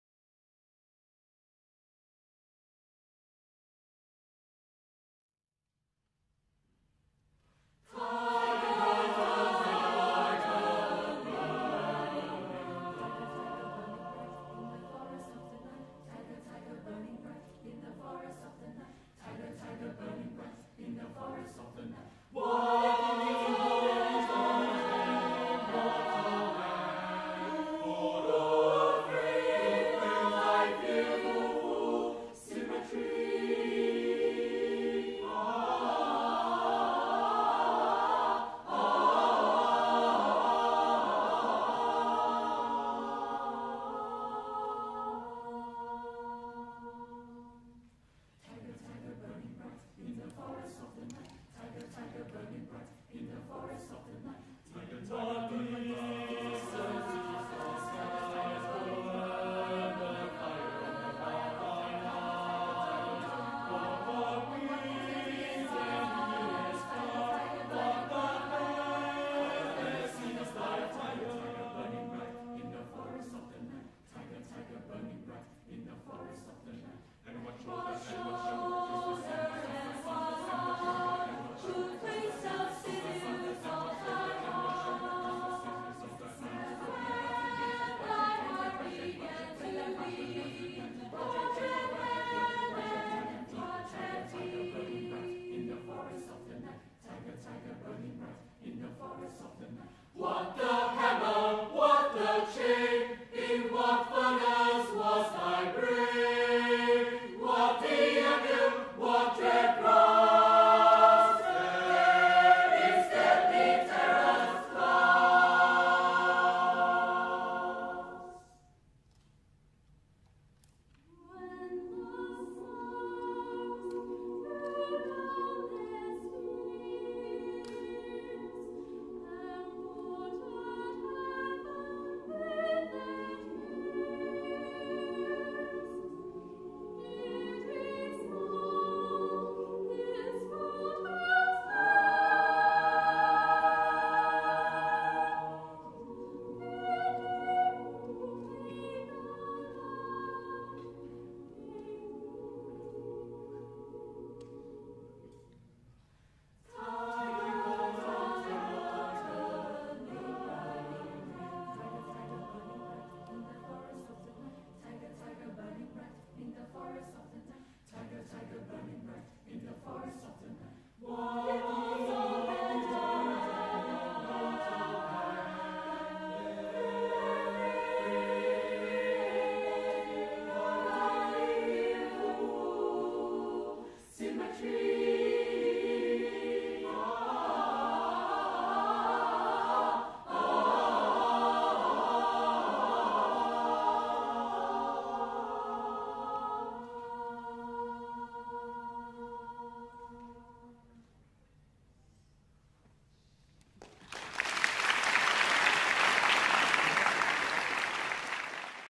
2013 - The Tyger
This poem describes The Tyger and its terrifying symmetry. The poem questions God’s intentions in creating the fearful and ferocious, when he can also create the gentle and kind.